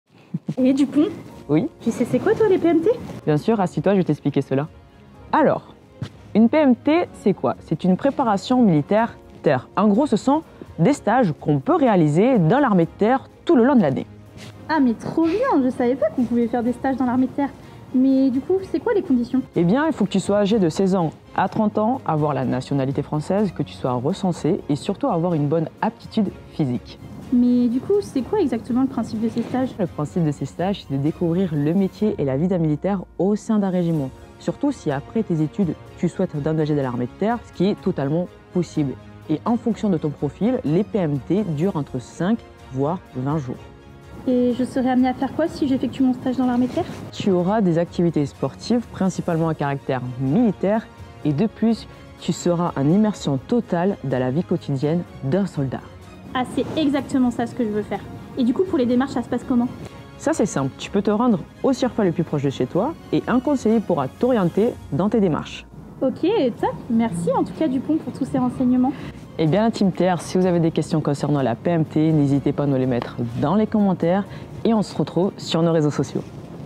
Préparations militaire
Savais-tu que l’armée de Terre propose aussi des stages ? Chez nous on appelle ça des préparations militaire !

Prépa montagne, prépa commando, prépa aéronautique… Il y a le choix.
On t’explique tout 👉